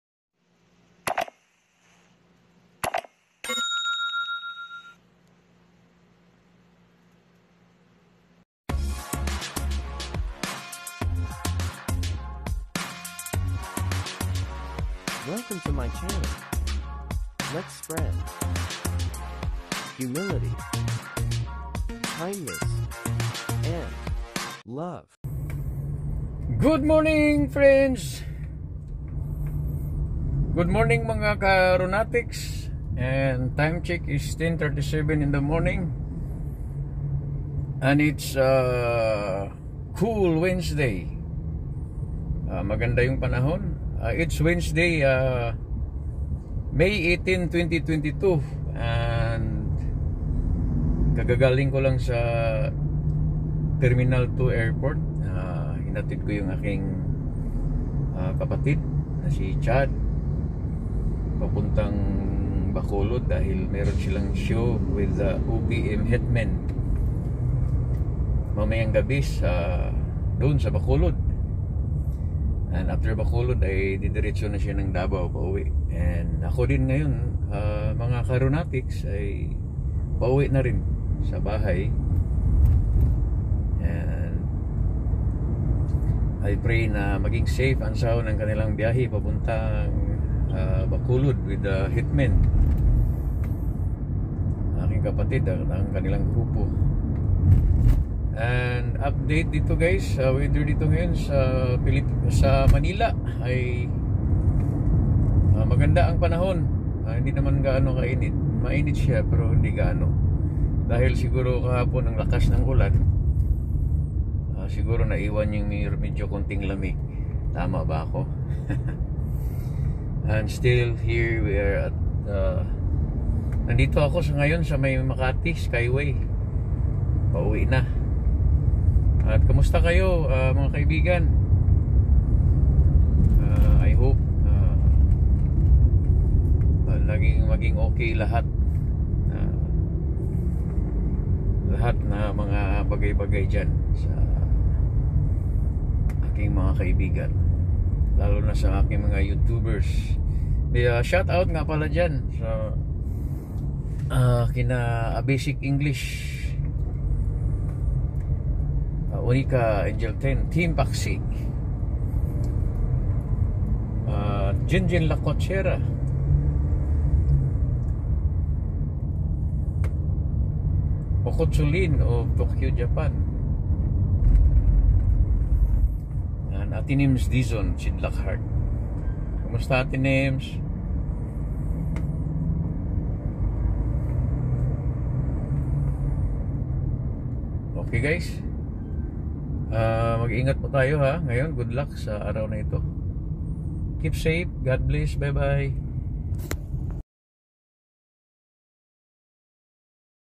Pauwi na ng bahay galing sa Airport Terminal 2 | Chad's Departure bound for Bacolod show
God is good | Happy Wednesday everyone